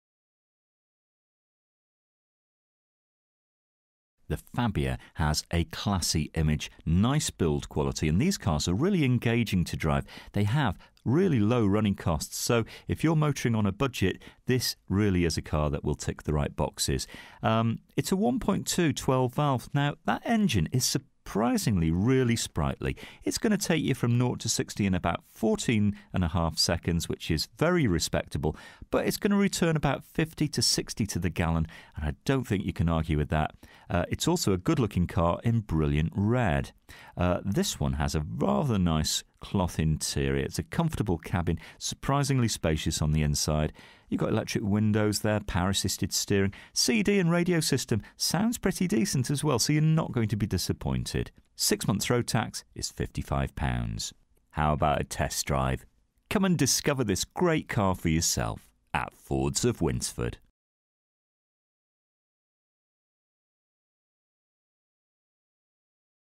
SKODA FABIA HATCHBACK (2010) 1.2 12V S 5DR - FD10JJV
DETAILS:
First registered: 27/05/2010
Current mileage: 19460
Registration: FD10JJV
Colour: RED

FEATURES:
front electric windows, remote central locking, cloth interior, all round airbags, pas, abs, steel wheels, solid paint, cd / mp3, foam kit, parcel shelf present, hand book present

This used car is for sale at Fords of Winsford Car Supermarket in Cheshire, UK